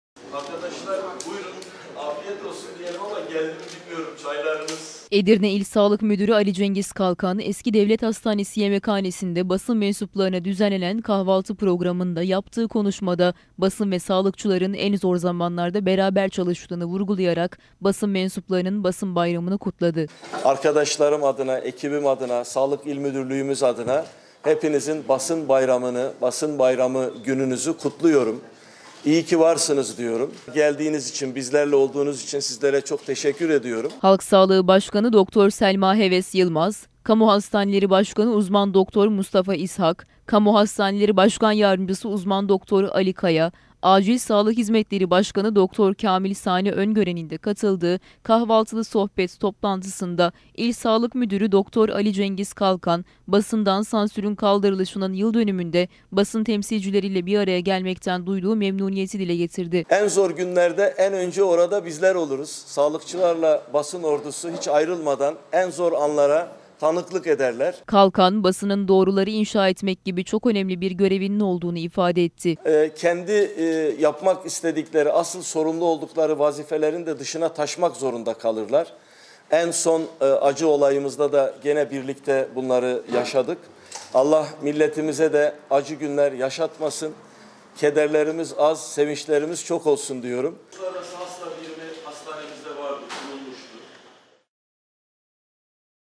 BASIN ÇALIŞANLARININ “BASIN BAYRAMI” KUTLANDI
Basında sansürün kaldırılışının yıldönümü olarak kutlanan 24 Temmuz dolayısıyla, İl Sağlık Müdürlüğü basın temsilcilerine yönelik kahvaltı etkinliği düzenledi.